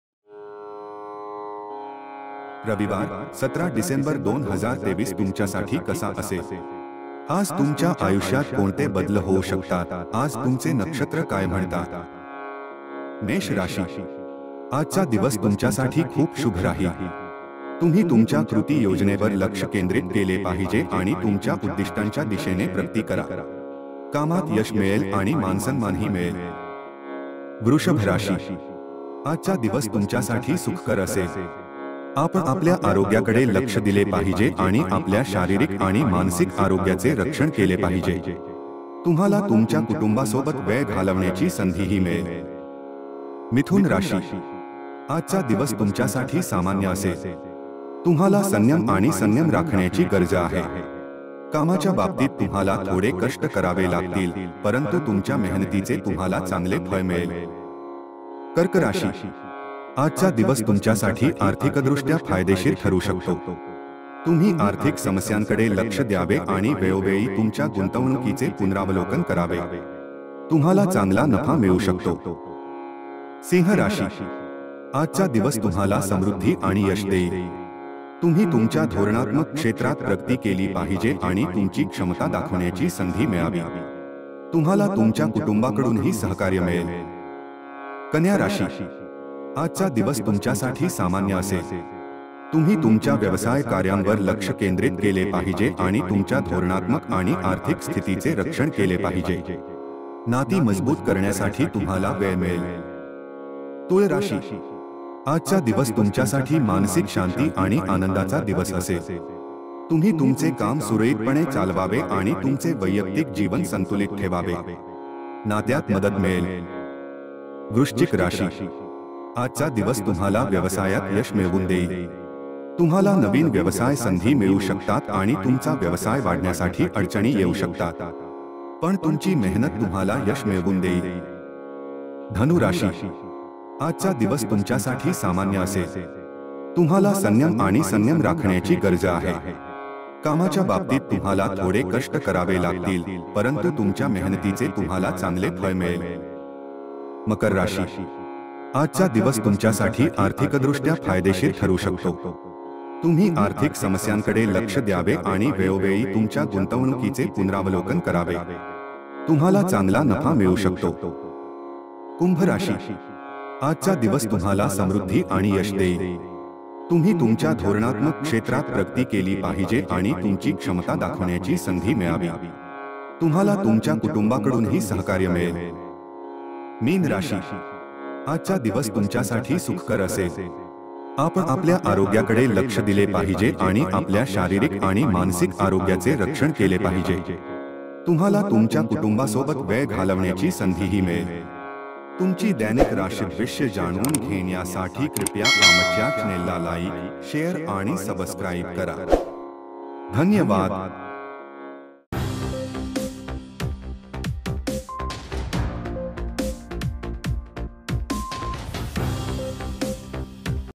राशीभविष्य | Aajche Rashi Bhavishya | Today's Horoscope | 17 डिसेंबर 2023
रविवार, 17 डिसेंबर 2023 तुमच्यासाठी कसा असेल? आज तुमच्या आयुष्यात कोणते बदल होऊ शकतात, आज तुमचे नक्षत्र काय म्हणतात.